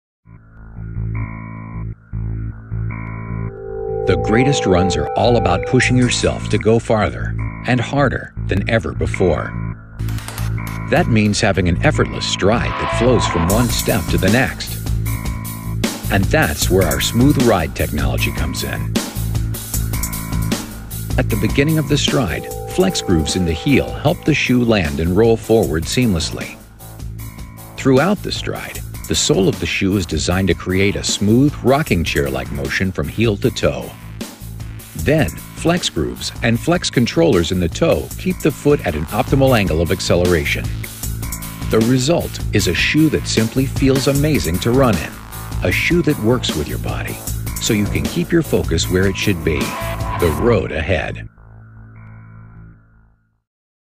Mizuno Smoothride Technology
Unique approach to the sole of the shoe designed to create a smooth, rocking-chair-like transition from heel to toe on every step.